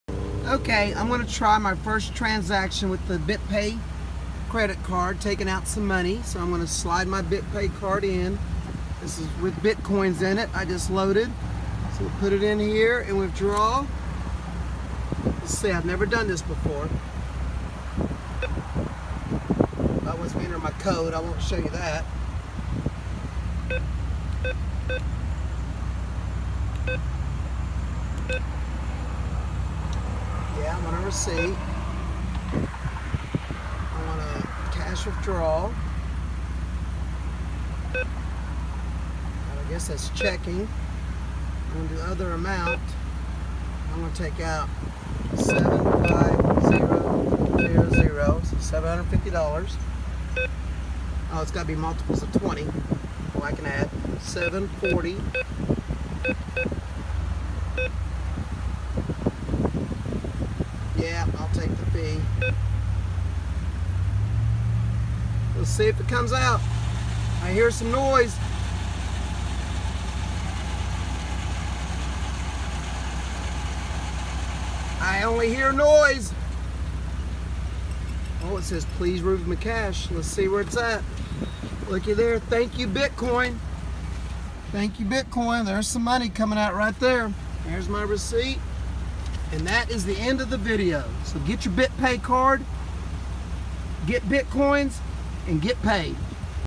BitPay Card Brad 1
Brad's first cash withdrawal funded by Bitcoins!!